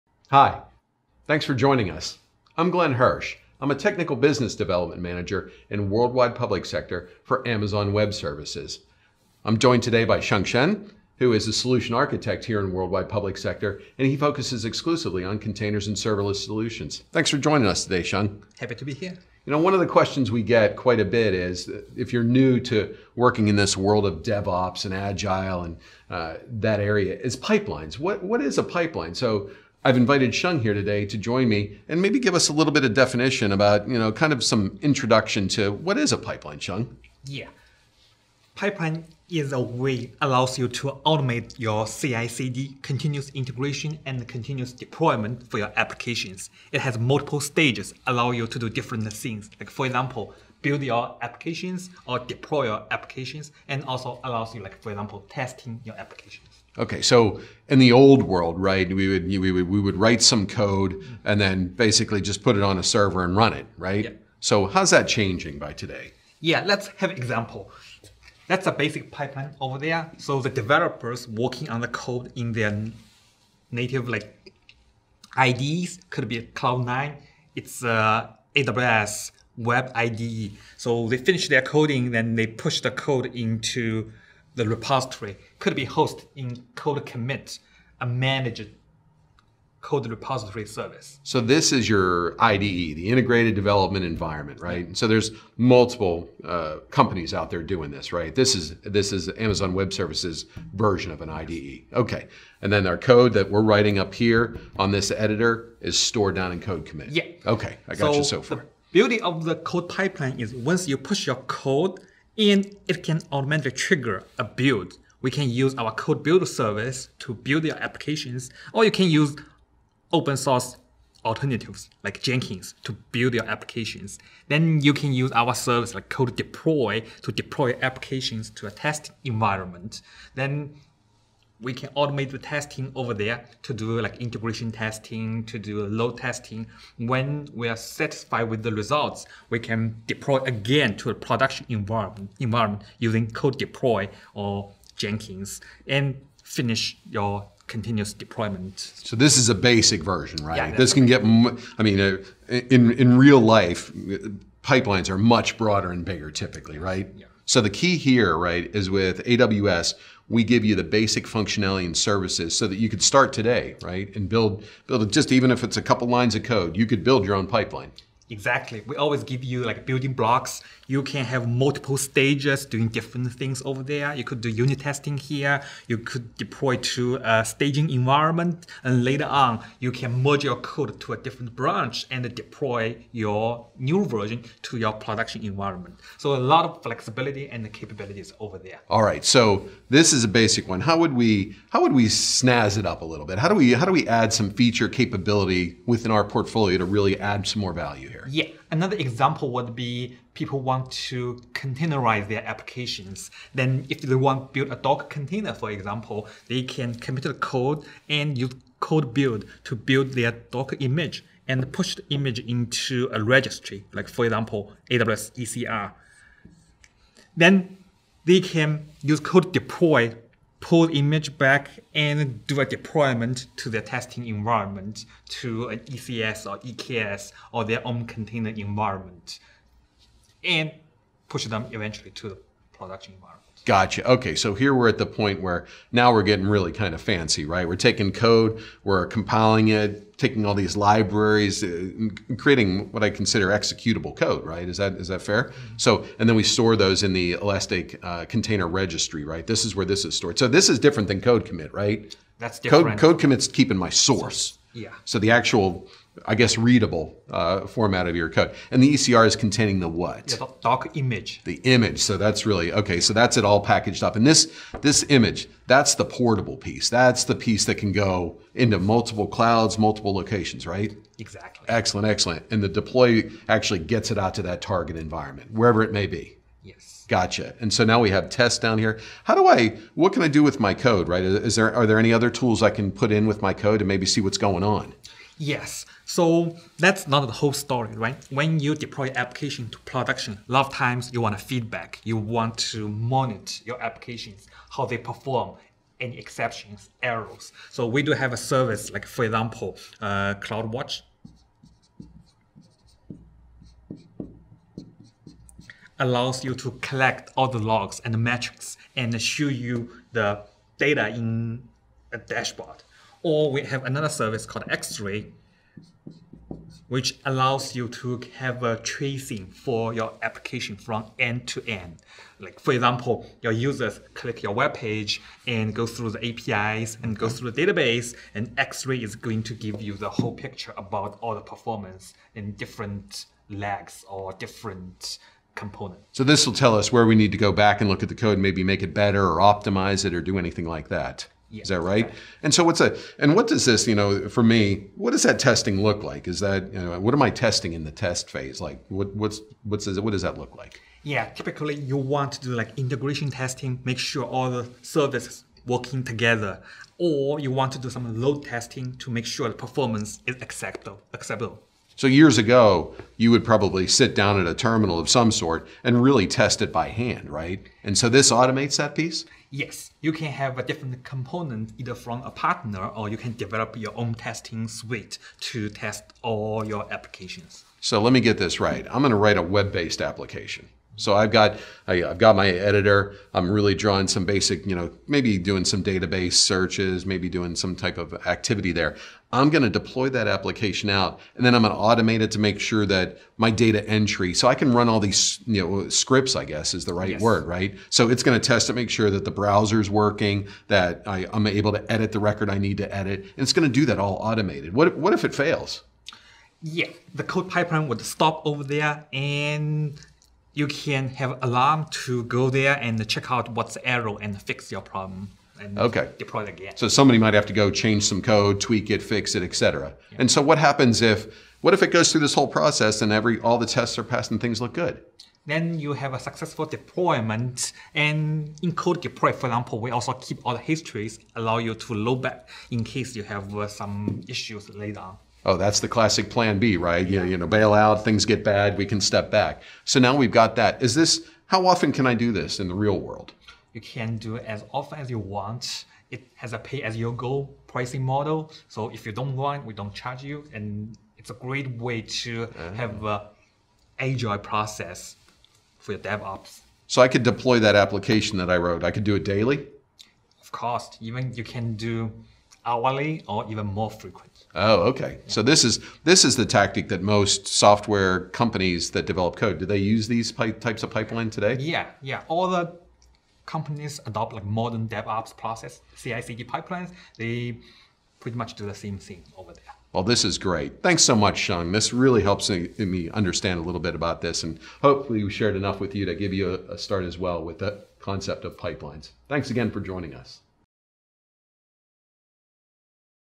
Using CI/CD Pipeline on AWS in Public Sector
Learn the fundamentals of building a CI/CD pipeline on AWS in government, education, and non-profit IT environments. We discuss the components of a pipeline using AWS CodeCommit, AWS CodePipeline, AWS CodeBuild, AWS CodeDeploy, and AWS Cloud9.